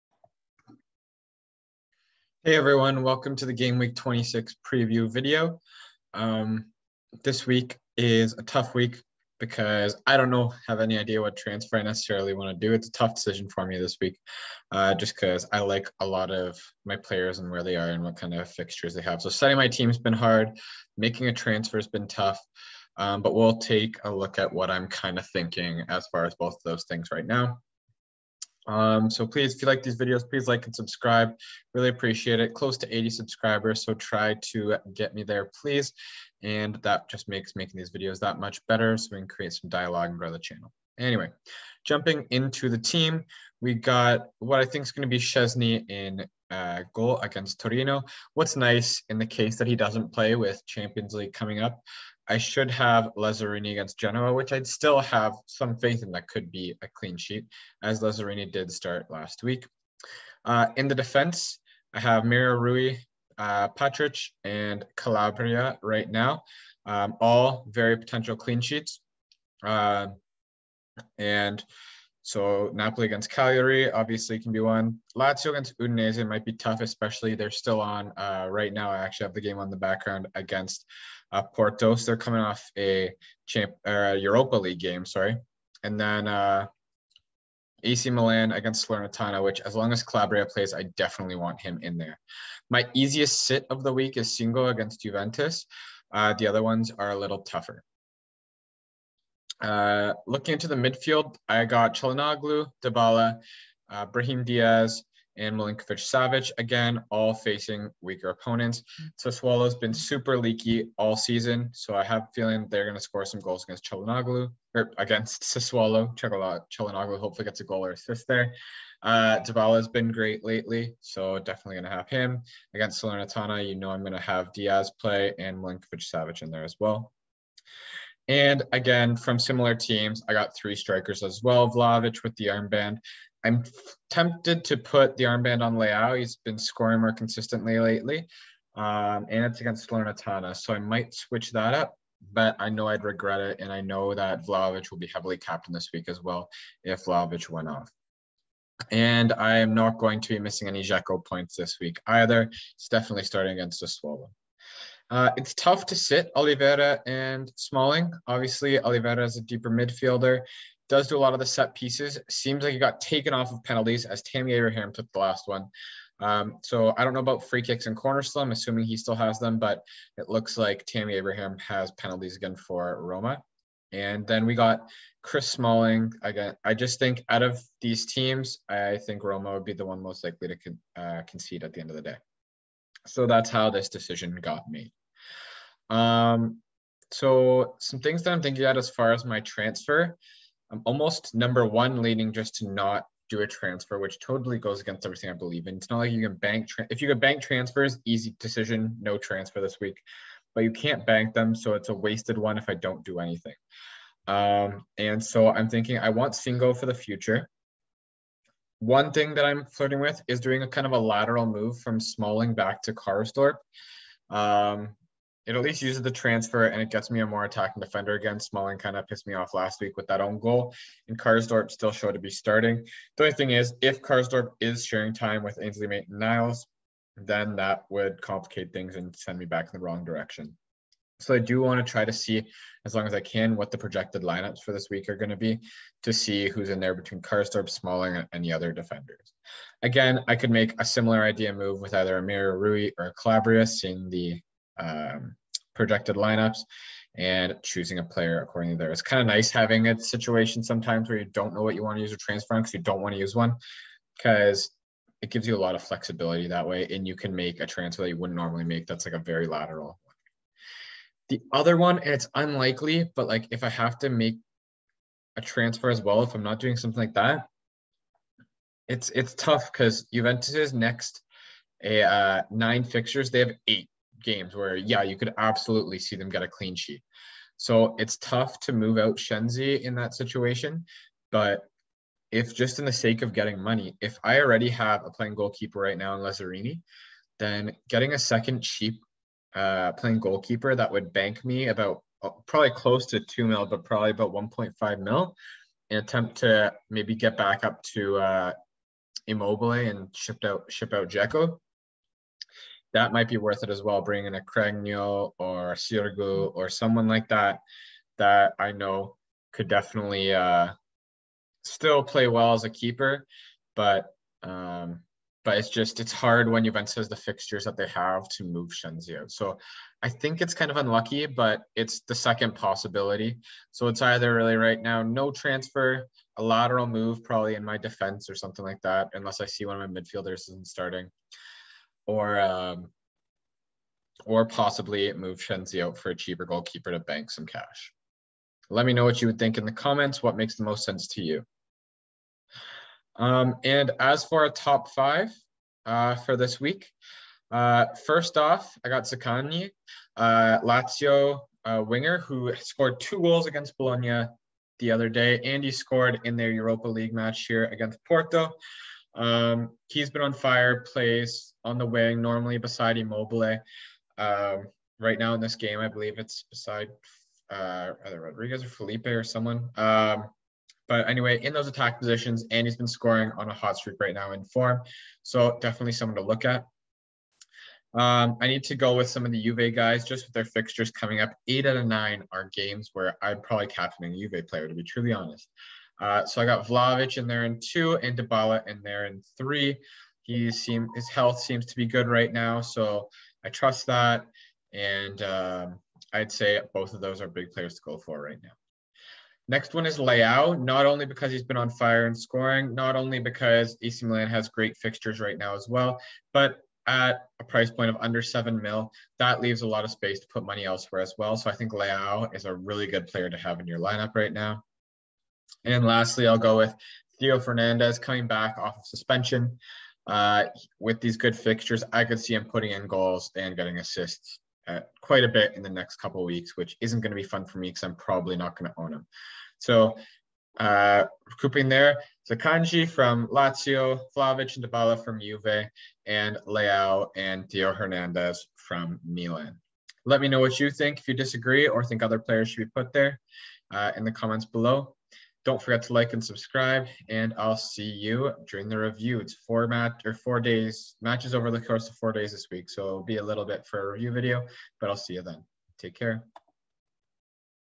Serie A Fantasy Gameweek 26 Preview: Transfer?!
Not sure who to transfer this Serie A Fantasy Gameweek.  What do you think is the play?